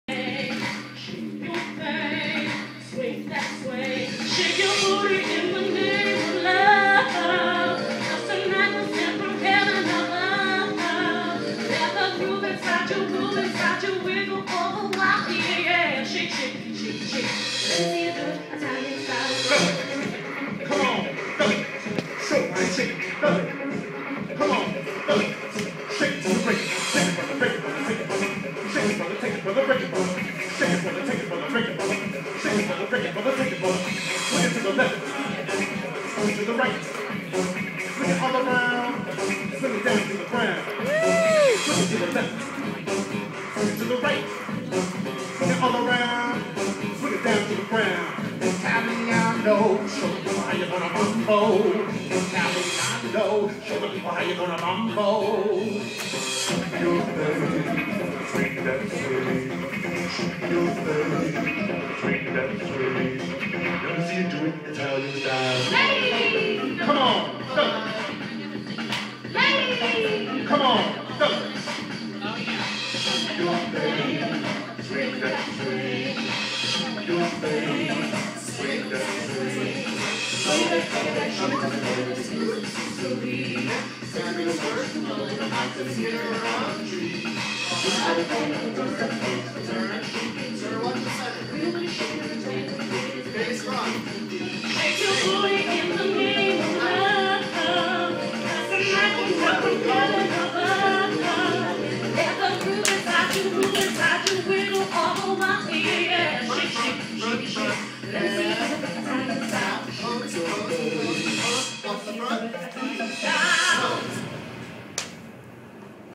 let me see you do it italian style ;]
Lauren and I rehearsing the 'Italian booty shake' for our guest appearance in 'My Big Gay Italian Wedding' with cast members Adam Zelasko and Liz Gerecitano